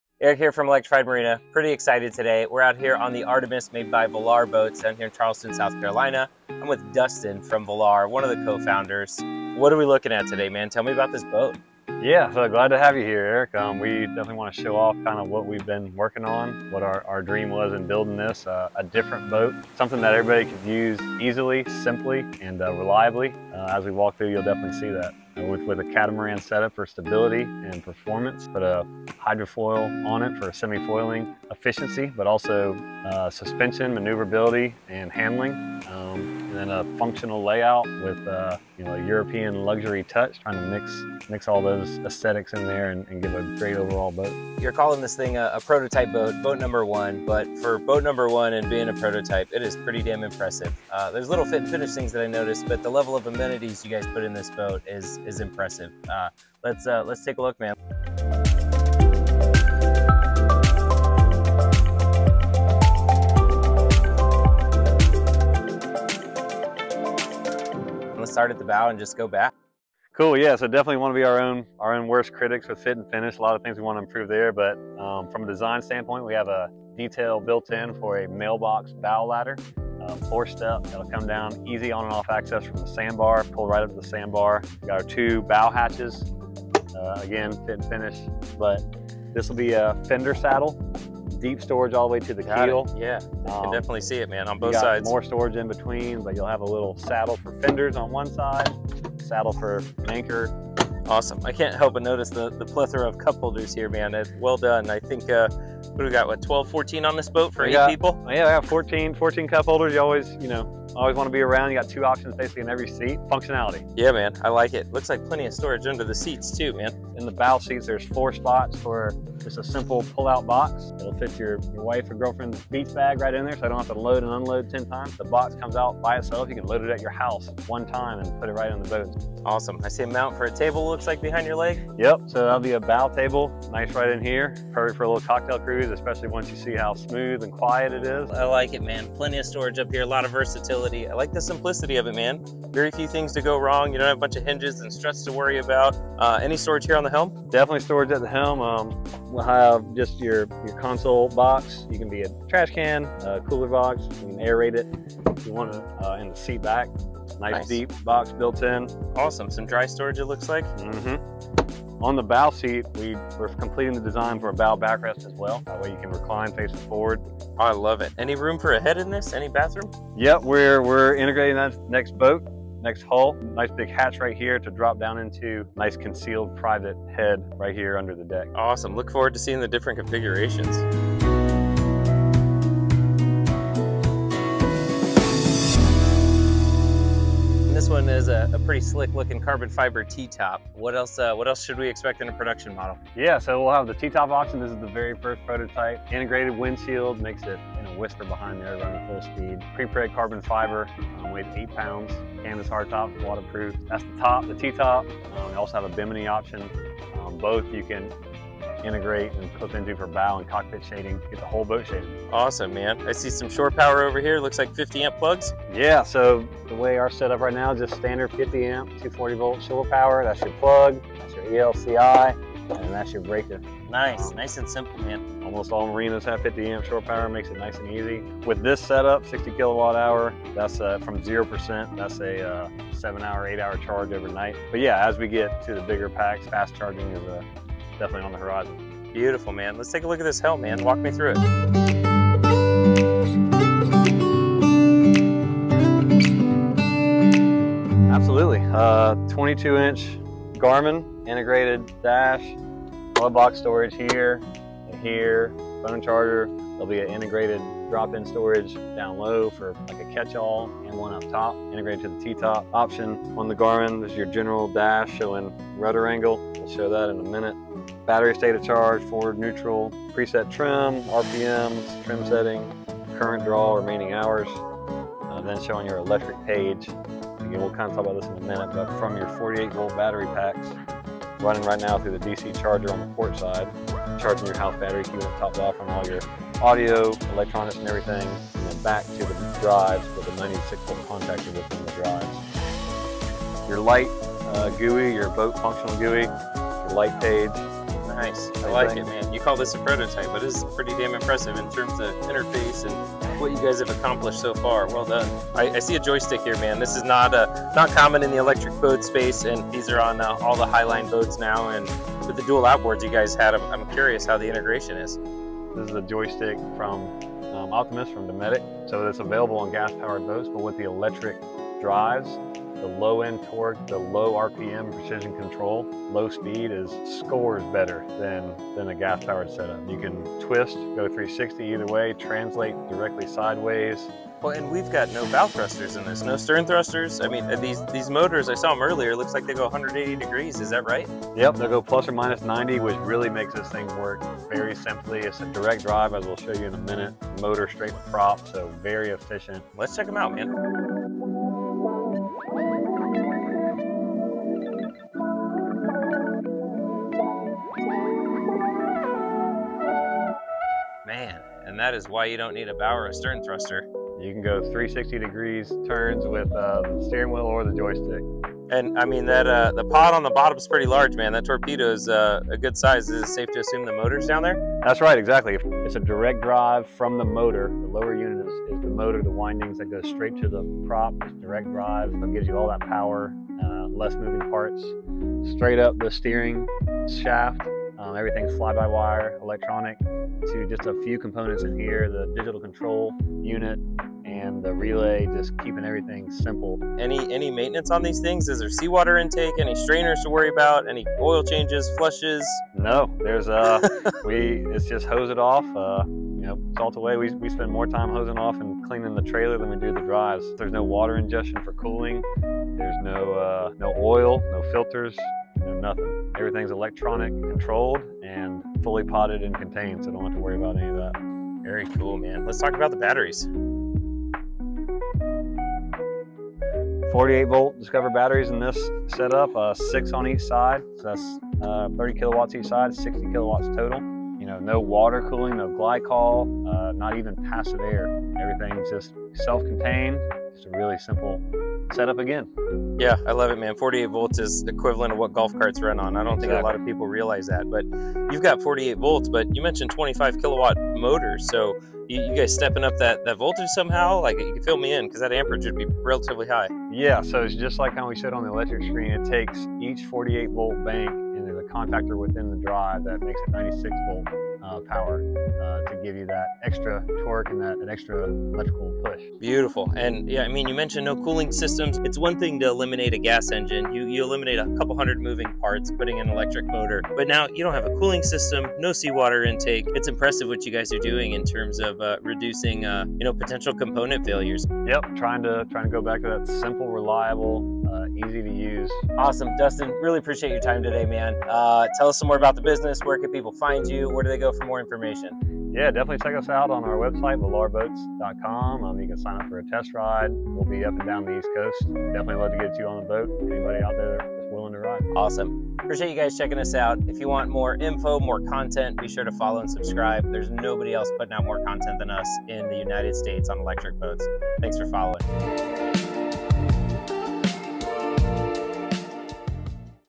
FLYING ELECTRIC CATAMARAN | 2025 Walkthrough of Artemis23
Erick met up with Dustin Tupper from Volare Boats in South Carolina to check out the first working prototype of the Artemis23. This next-generation electric catamaran uses advanced hydro-assist technology to reduce drag, extend range, and deliver a smoother, more efficient ride than traditional electric boats.

The Artemis23 is shaping up to be one of the most exciting breakthroughs in the electric marine industry, combining cutting-edge efficiency tech with modern design, comfort, and real-world performance. Whether you’re researching electric boats, curious about hydrofoil and hydro-assist systems, or looking for the next big innovation in sustainable boating, this walkthrough is packed with details you won’t want to miss.